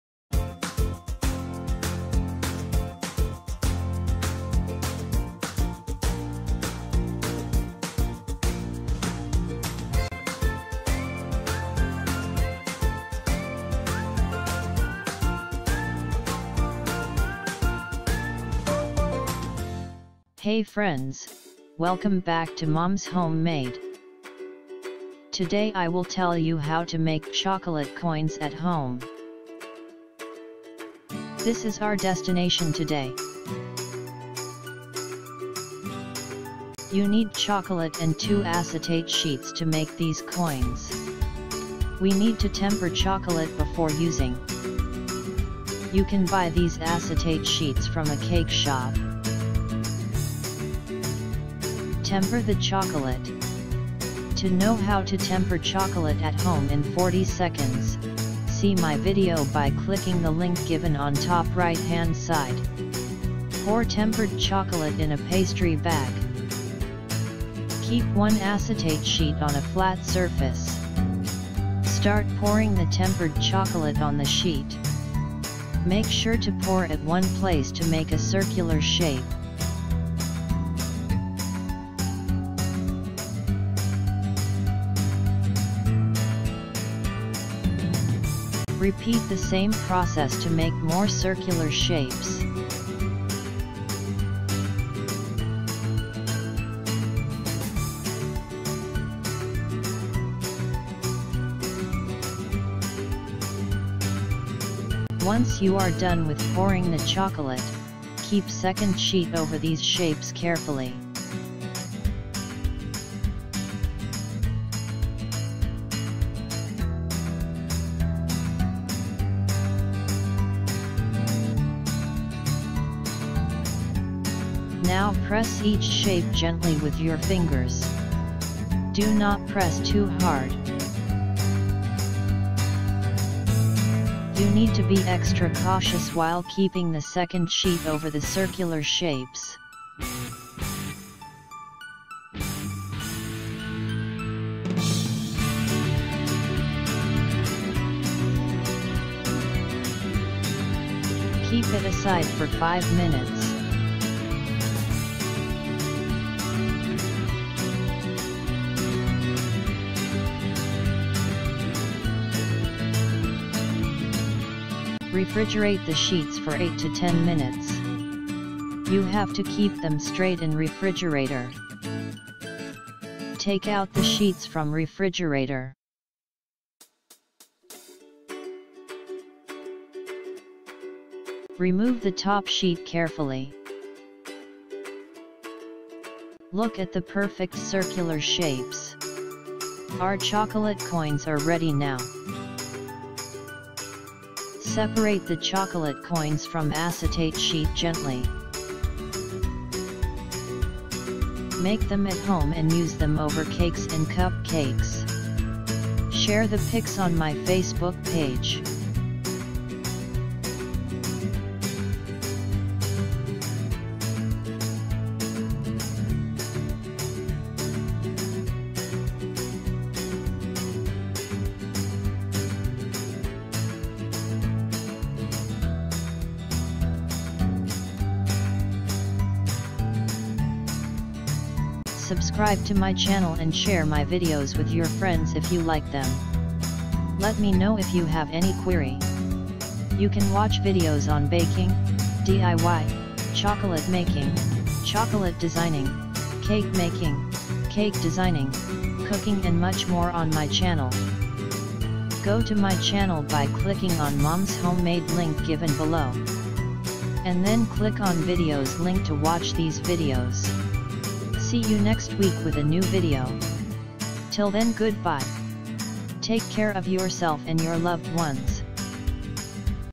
Video #87 -How to make chocolate coins
How to make chocolate coins @home

See more chocolate videos on  my channel by clicking below link